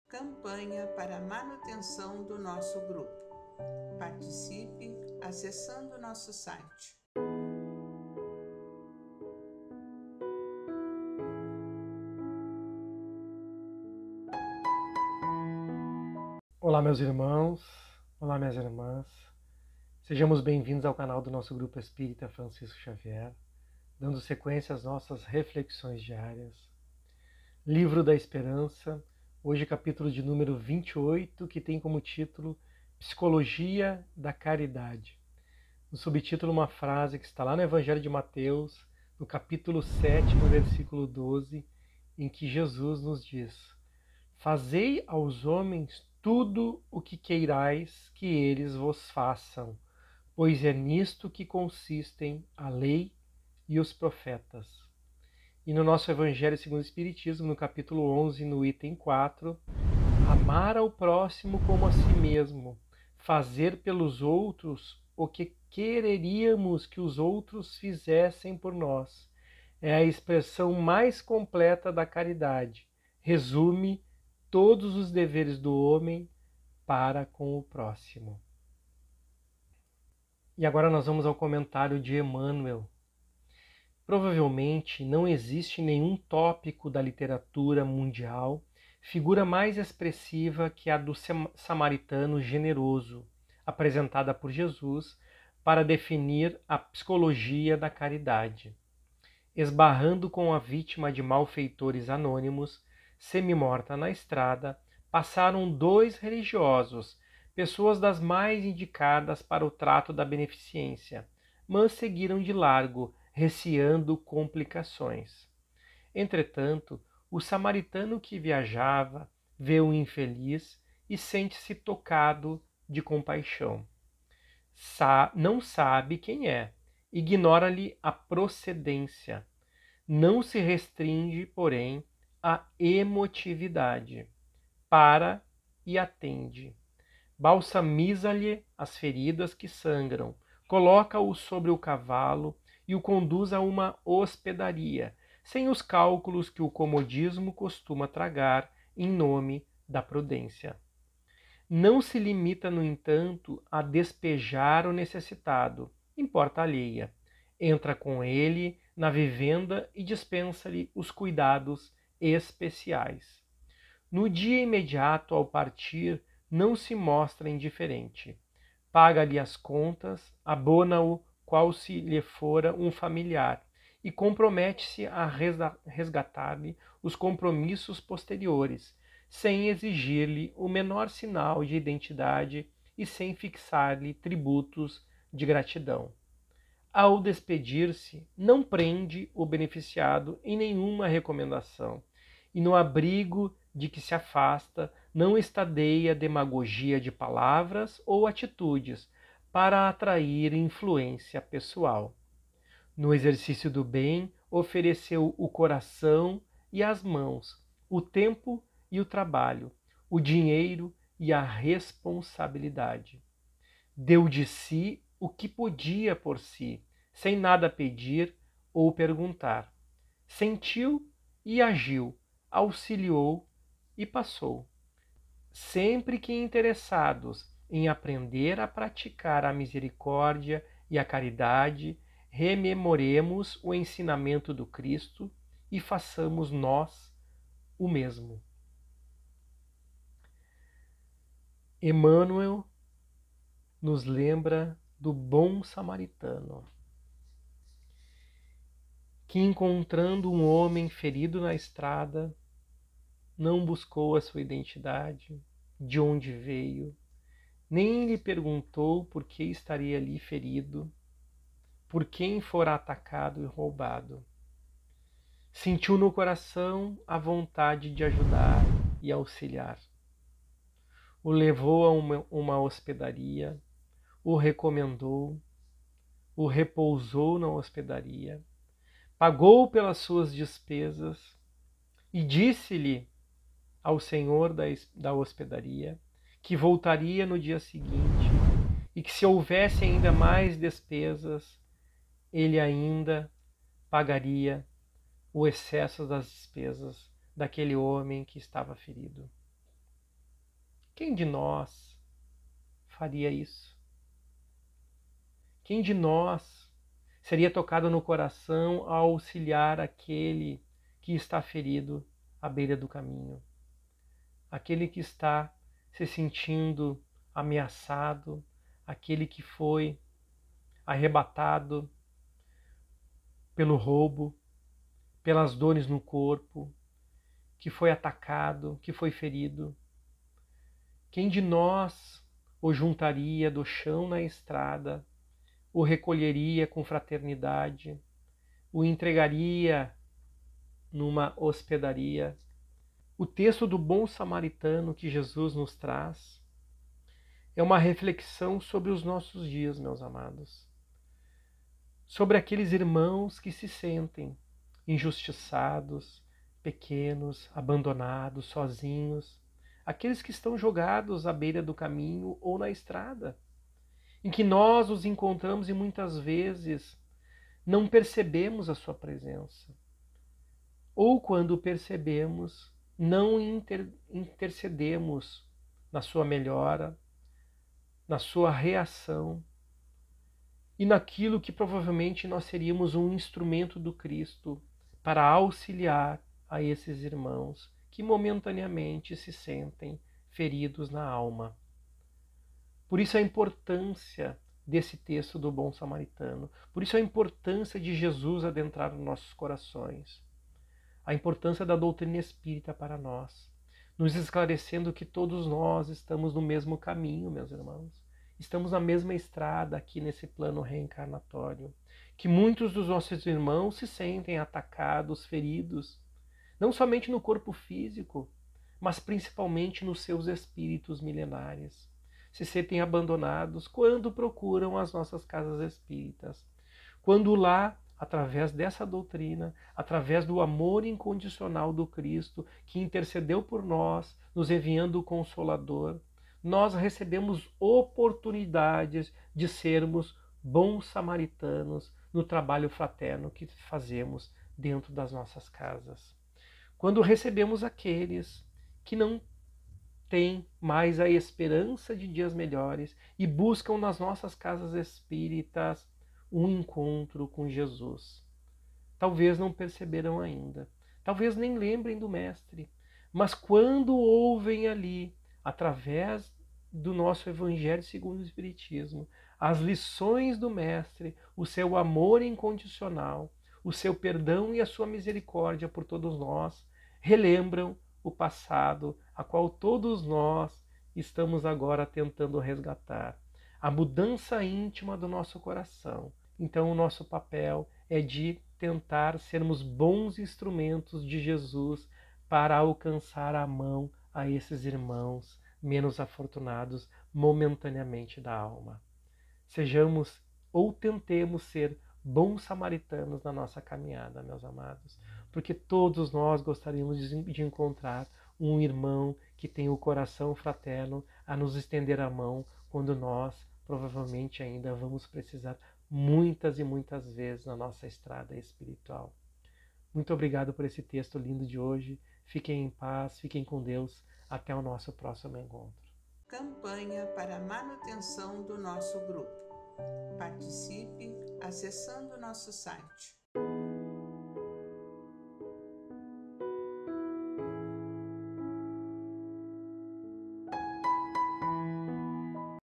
Psicologia da caridade - Livro da Esperança - José Henrique Gomes
Reflexões diárias, por José Henrique Gomes, Trabalhador do Grupo Espírita Francisco Xavier, texto retirado do Livro da Esperança, psicografado por Francisco Cândido Xavier, pelo Espírito Emanuel, Cap. 28 Psicologia da caridade.